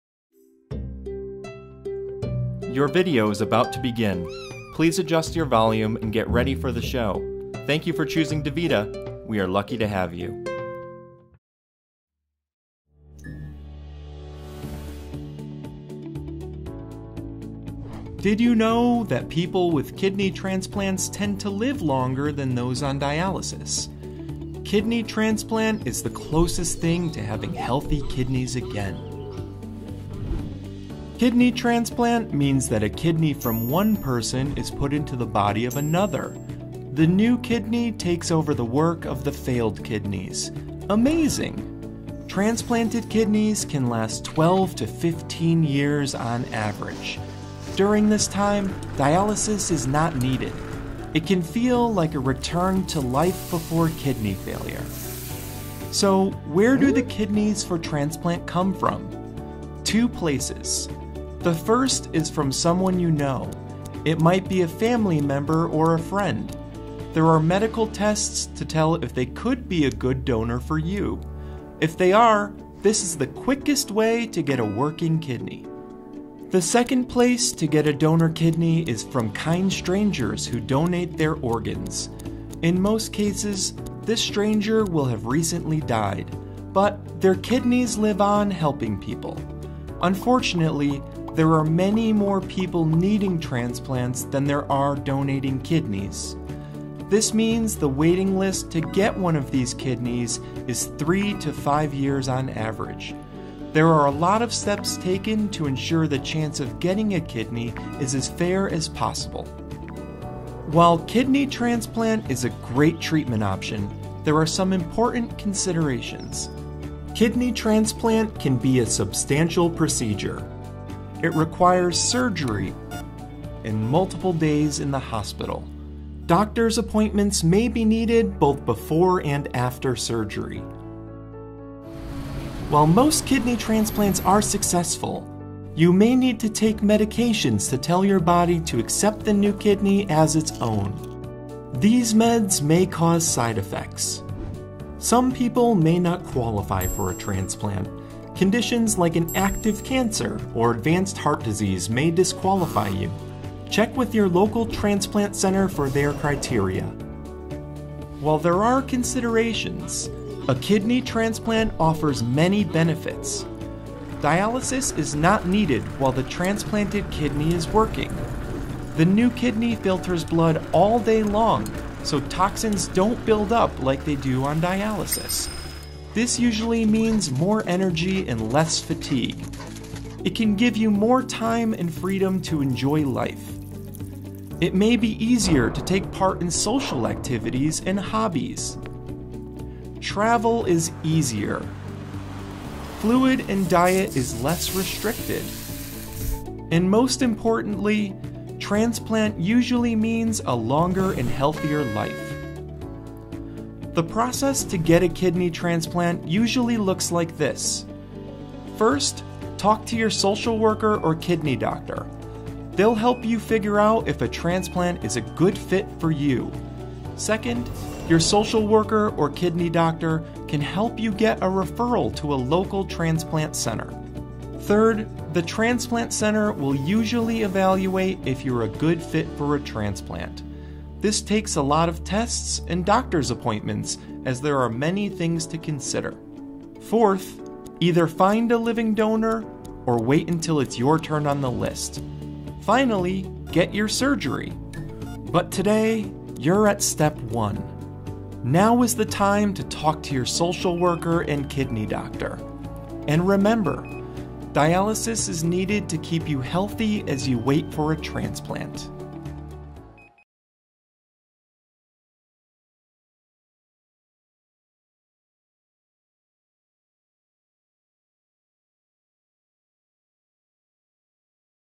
An Introduction to Kidney Transplant
A kidney transplant can be a viable treatment option for many people with chronic kidney disease. Learn about how the kidney transplant journey starts and the potential benefits of this treatment choice.